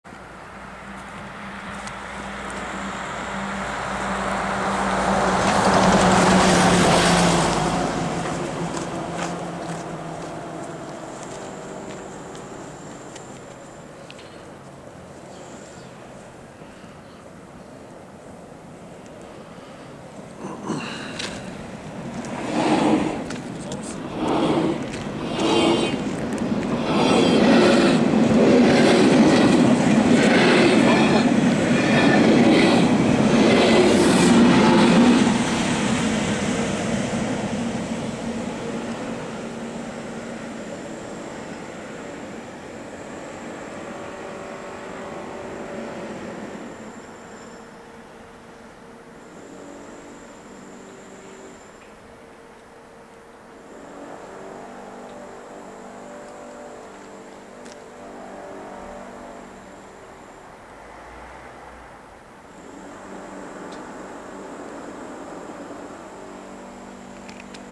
IR Engelberg - Luzern kurz nach der Ausfahrt aus dem Bhf Engelberg
Zentral Bahn (ZB). Ex Luzern-Stans-Engelberg Bahn (LSE) und ex. Brünigbahn (SBB).